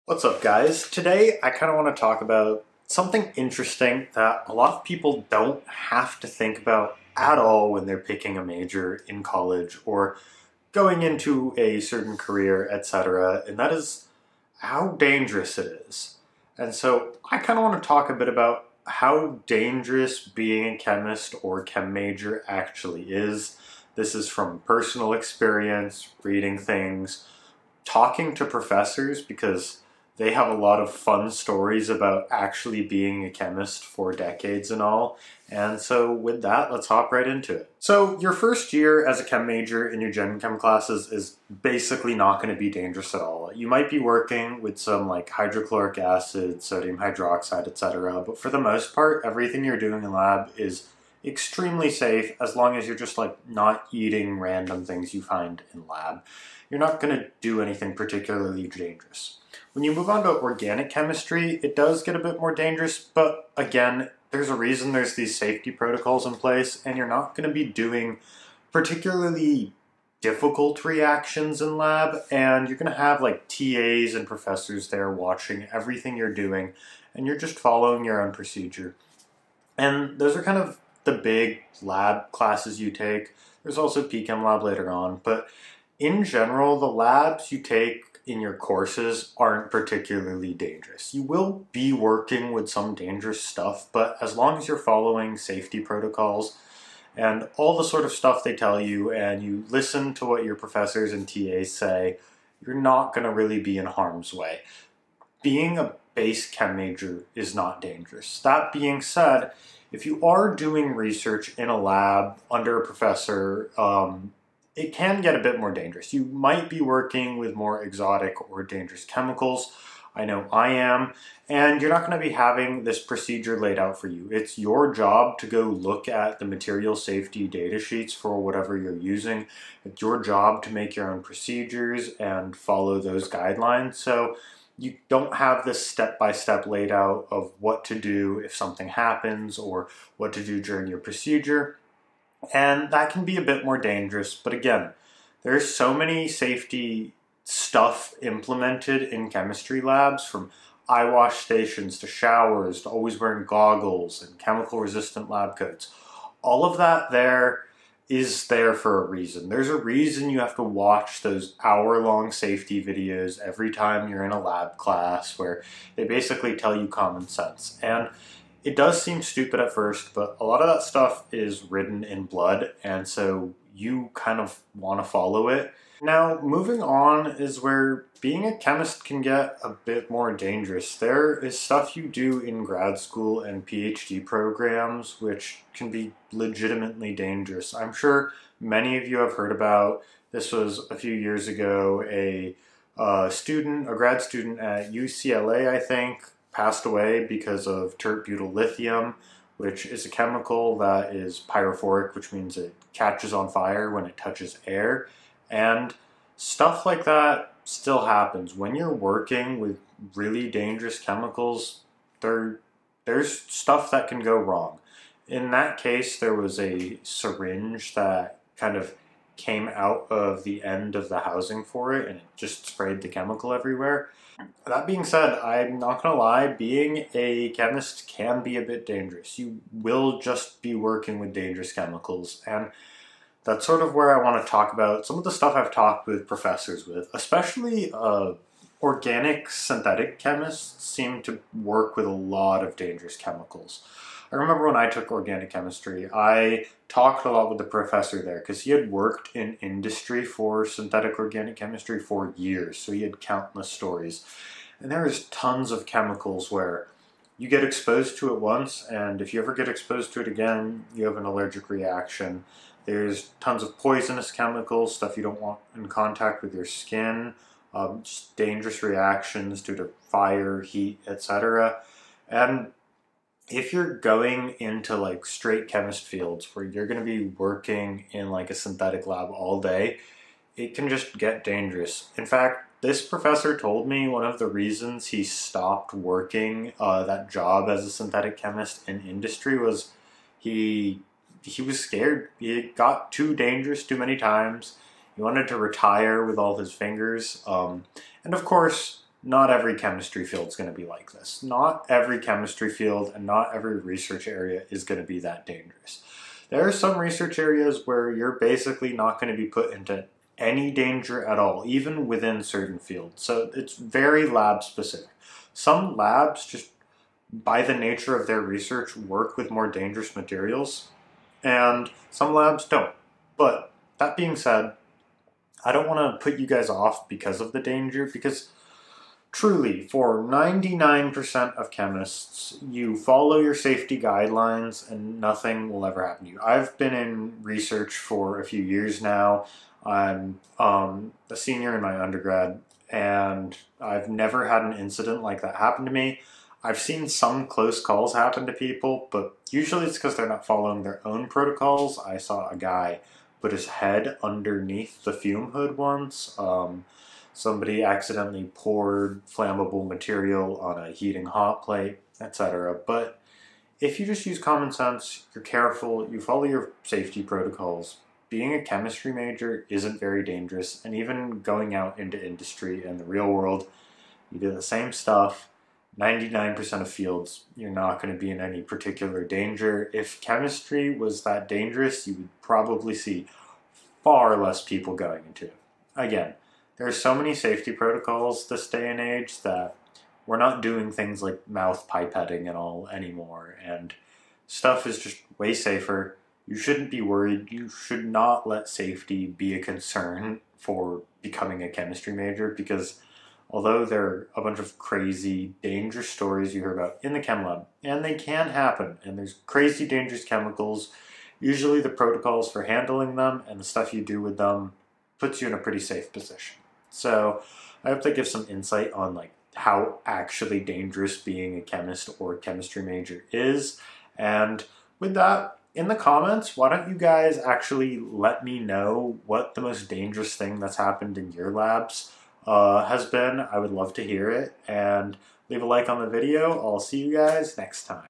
How Dangerous Is Chemistry Actually?
We hear about dangerous chemicals all the time and these types of chemicals are what chemists have to work with on a daily basis. Because of this you may think that being a chemistry major is actually really dangerous. In this video I talk about how dangerous it actually is to be a chemistry major or chemist and why it shouldn't dissuade you from the major.